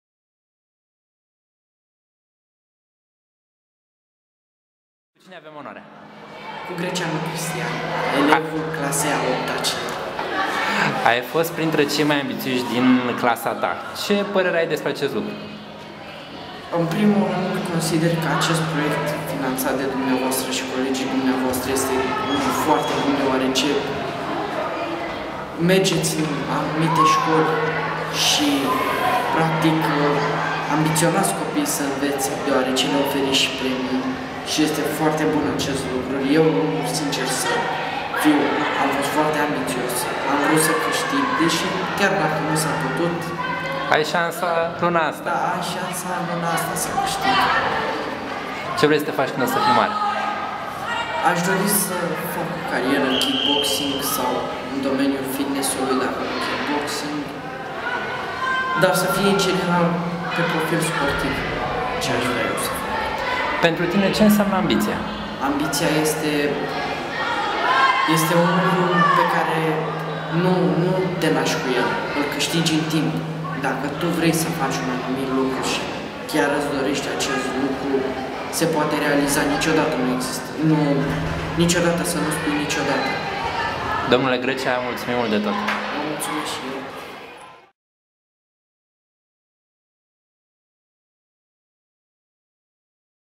Dl. Greceanu, Scoala Jose Martin-Bucuresti, despre Bursele StartEvo
Cristian Greceanu, Scoala Jose Martin-Bucuresti, despre Bursele StartEvo.